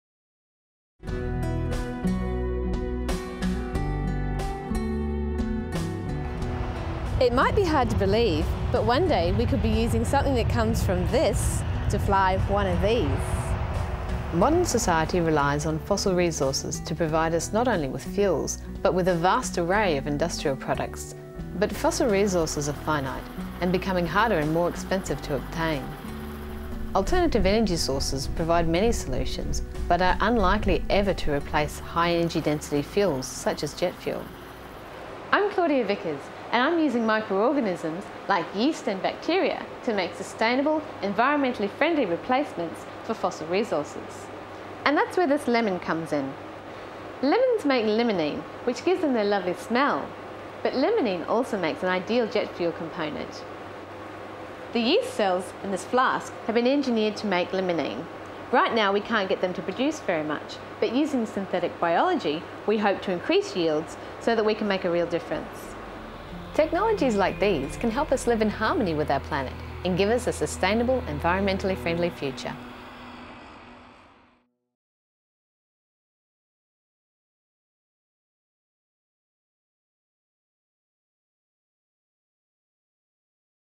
From a petrochemical to a biochemical economy
AIBN's Dr Claudia Vickers has received a UQ Research Excellence Award and $80,000 for her project, From a petrochemical to a biochemical economy. Dr Vickers is using synthetic biology to produce sustainable, environmentally friendly fuels and chemicals from engineered microbial cell bio-factories. She is engineering yeast cells with improved bio-conversion of sugar, and testing the technology on production of bio-jet fuel.